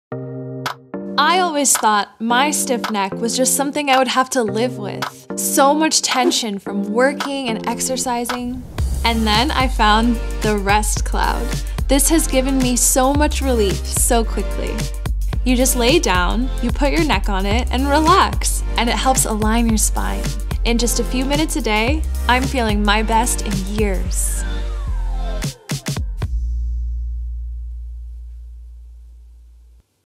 Neck and Shoulder Relaxer Device
Click on link for more information and buy product
RESTCLOUD Neck and Shoulder Relaxer, Cervical Traction Device for TMJ Pain Relief and Cervical Spine Alignment, Chiropractic Pillow, Neck Stretcher(Pink)
Relief neck pain in just 10 minutes.
Simple and effective physical solutions to sooth away stiff neck, helps in restoring proper cervical curvature associated with consistent use.
Dense and soft foam design provides sturdy, lightweight, and comfortable base.
Can't be used as an ordinary pillow all night long.
Normally you will need 1-3 days to adapt to this pillow, because your neck needs time to familiar with the new corrector curvature. You will enjoy an extreme comfort after you get used to it!